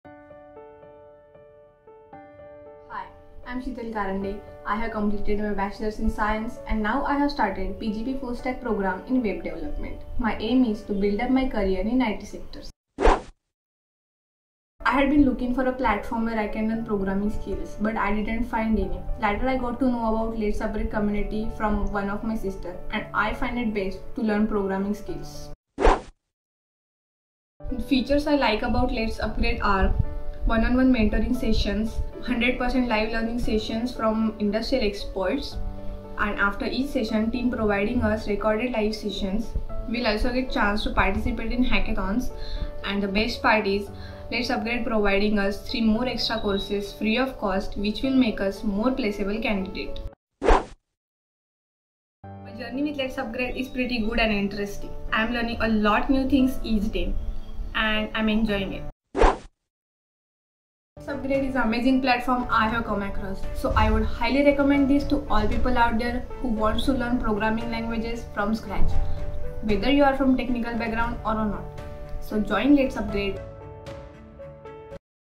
LetsUpgrade's Full Stack Developer in the making 🚀 - Shital Karande
Shital Karande is a top-notch developer in the making, with LetsUpgrade's Full Stack Program, she is shaping her developer career to the next level.

Timeline:
0:00 Who is Shital?
0:13 Why did you join LetsUpgrade
0:25 Things you like about LetsUpgrade
0:50 Journey with LetsUpgrade
0:59 A message from Shital to you